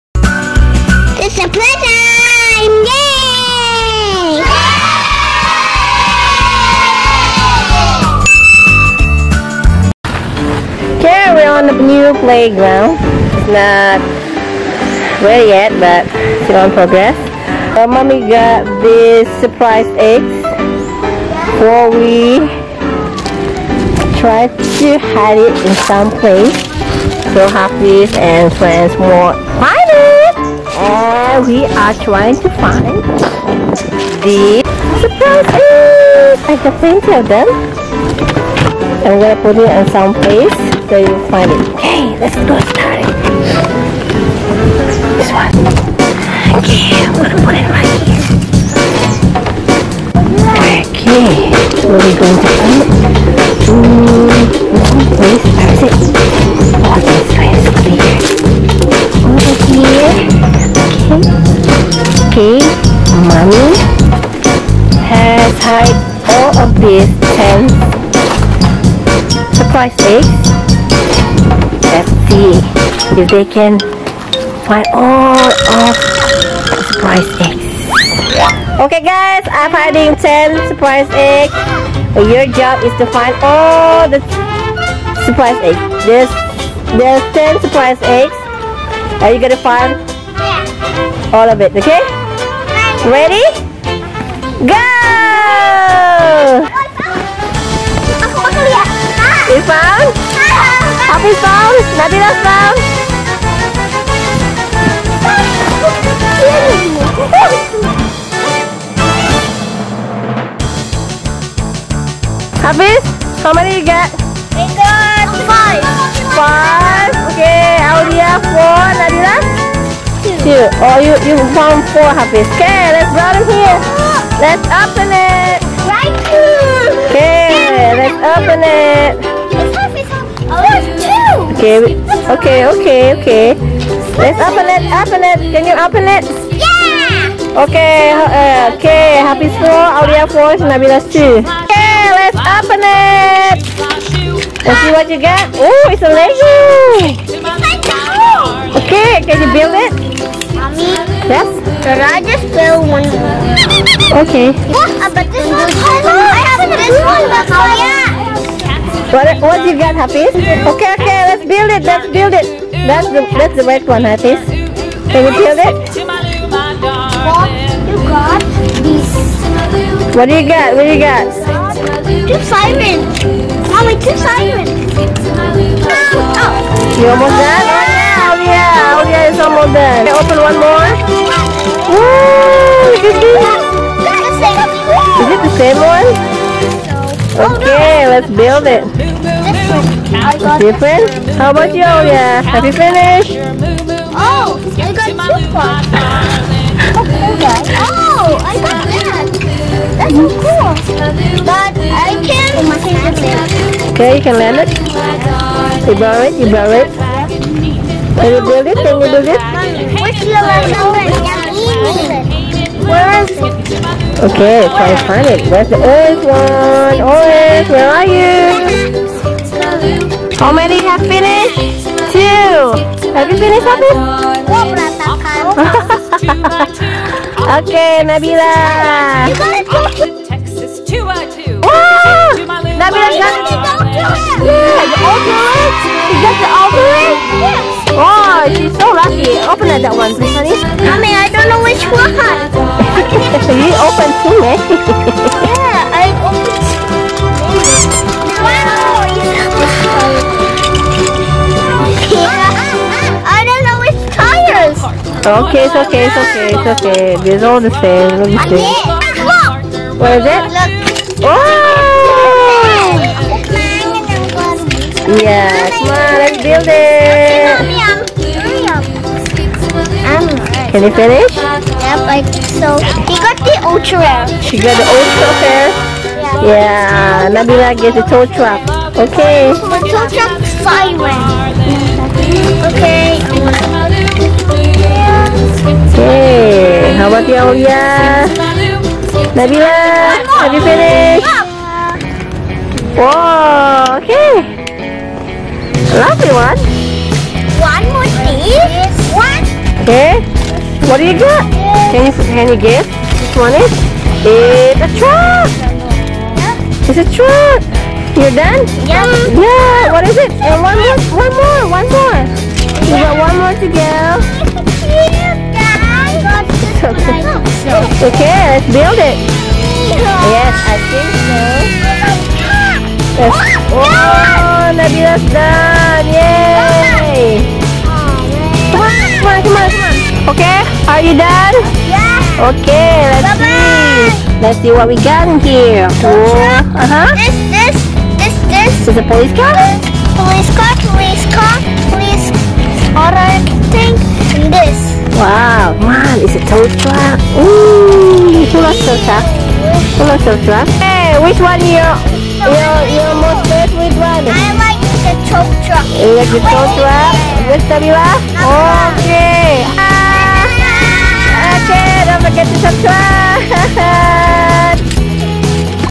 Hafiz Play Surprise Eggs Hunt & Open Toys
Hafiz and friends pretend play with surprise eggs hunt and open toys. Hunting the hidden surprise eggs from mommy.